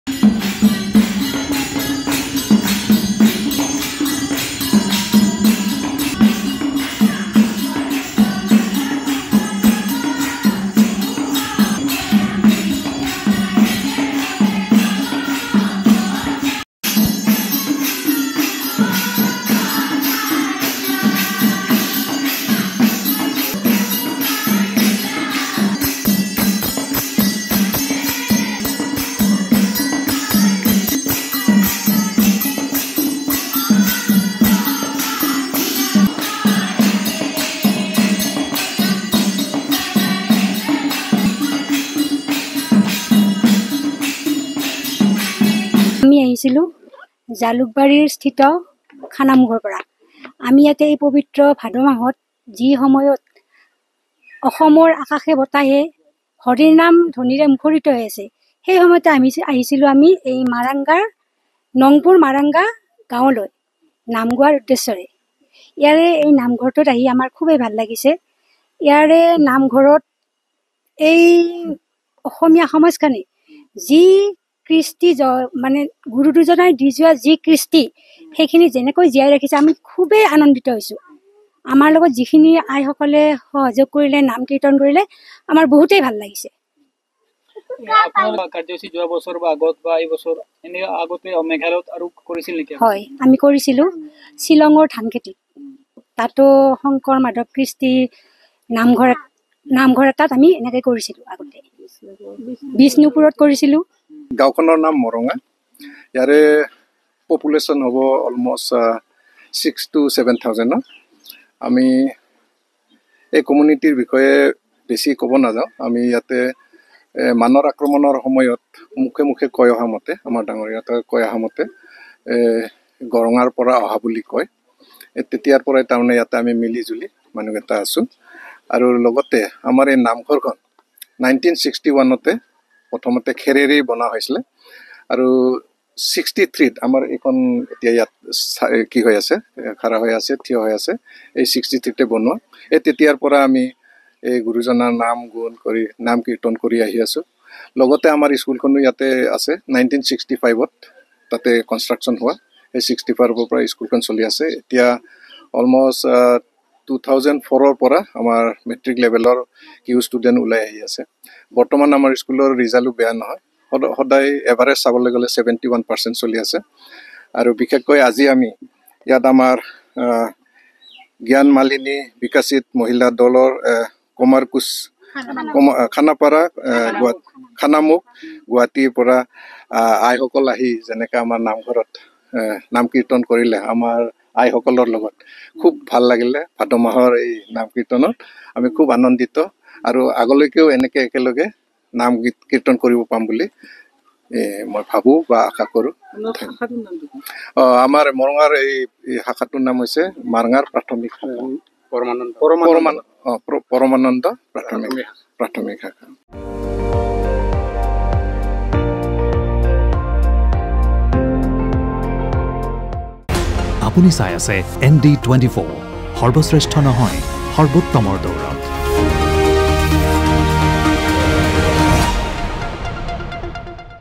Assam News: চুবুৰীয়া ৰাজ্য মেঘালয়ৰ নংপোৰ মাৰাংগাৰত চলি আছে এমহীয়া ভাদ মাহৰ নাম
চুবুৰীয়া ৰাজ্য মেঘালয়ৰ নংপোৰ মাৰাংগাৰত চলি আছে এমহীয়া ভাদ মাহৰ নাম। পৰিস্থিতিৰ তাগিদাত পৰি বহিঃ ৰাজ্যত থাকিলেও এতিয়াও শিপা এৰি দিয়া নাই তাৰ বাসিন্দা সকলে। অসমৰ পৰা দূৰত থাকিলেও তেওঁলোকে নীৰৱে চৰ্চা কৰি আহিছে অসমীয়া সংস্কৃতিৰ।